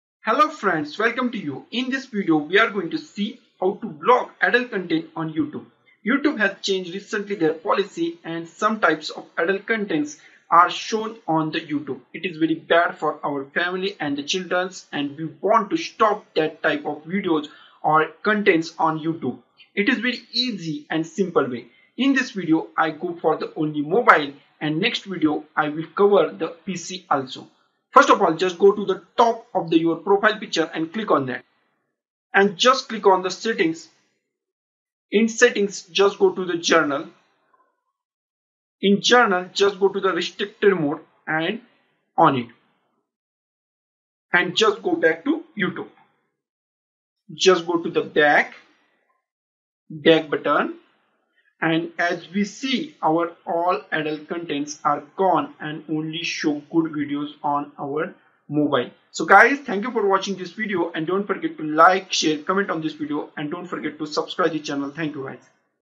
How to Block Adult Video Content on YouTube
YouTube has  recently changed their policy of  adult content  and   now on YouTube videos  many  adult,  pornographic, porn videos, bad content is  appearing.    we are a family  person  to protect our family from  this kind of  bad videos  you has to stop 18 plus videos. In this video I will show you how to block those types of videos.